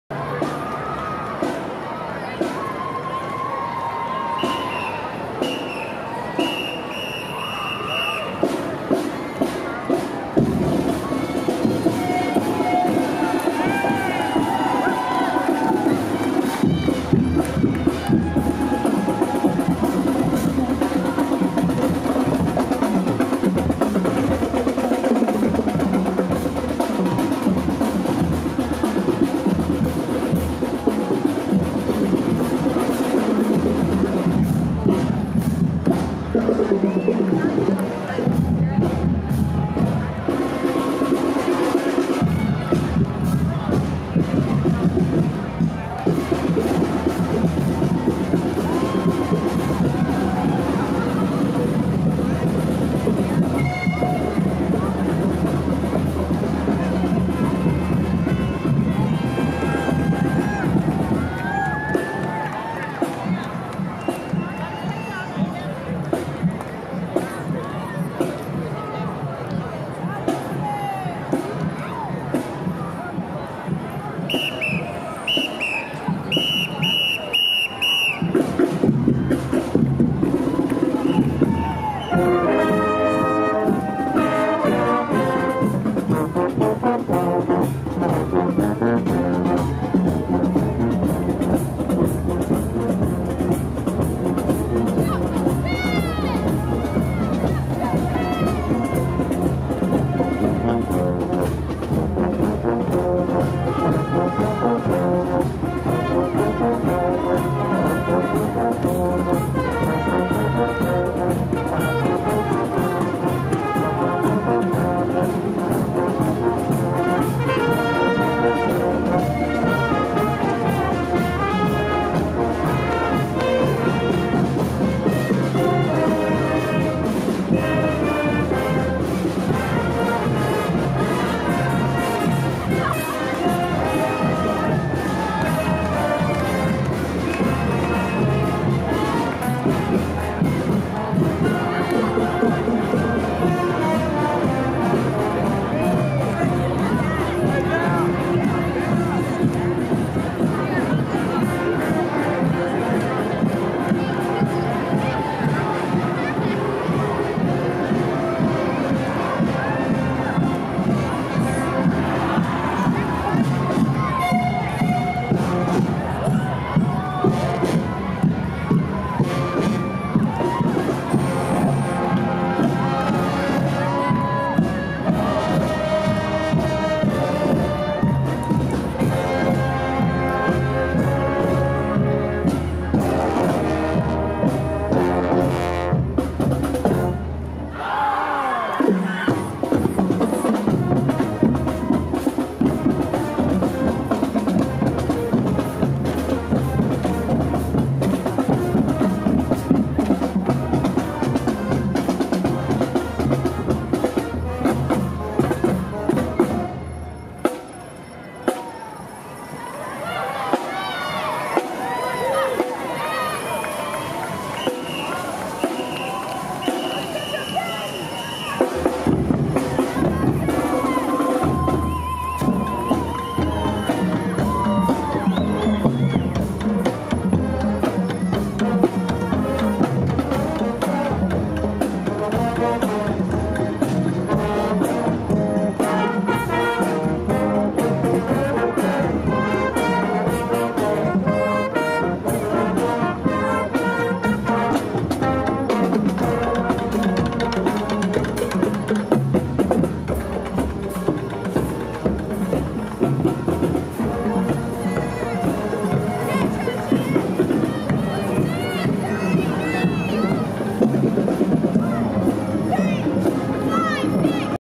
Wilson High School Marching Band
The Wilson High School marching band performing in the Rose Festival Starlight Parade.  Portland, OR, June 4 2016.